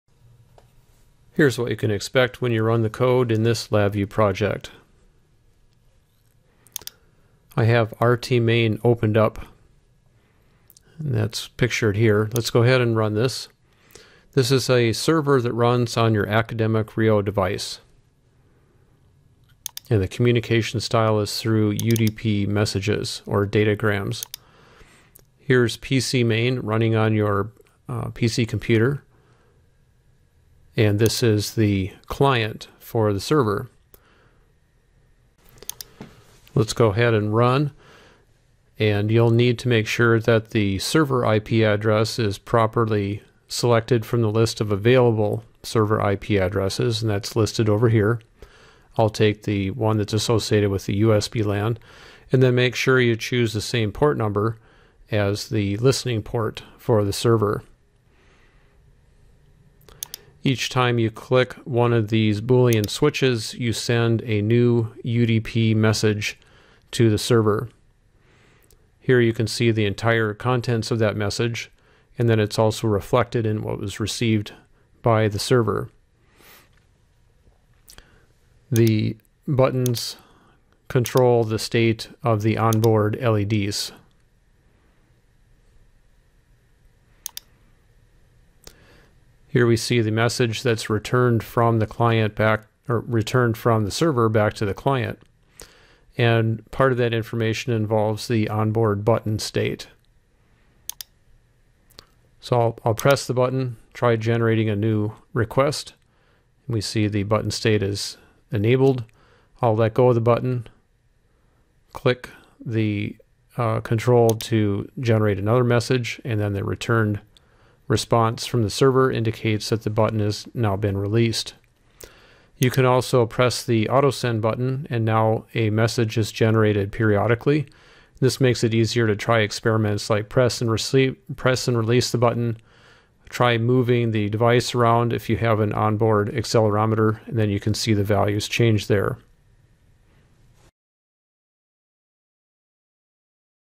LabVIEW code: UDP client-server (expected results)
Operating instructions and expected results for the "rt_udp-client-server" LabVIEW project available for download that covers this topic: "Create a server on the Academic RIO Device that listens for UDP datagram messages from a client running on the PC host, accepts client information including the desired state of the four onboard LEDs, sets the LEDs accordingly, and returns the state of the onboard 3-axis accelerometer and pushbutton."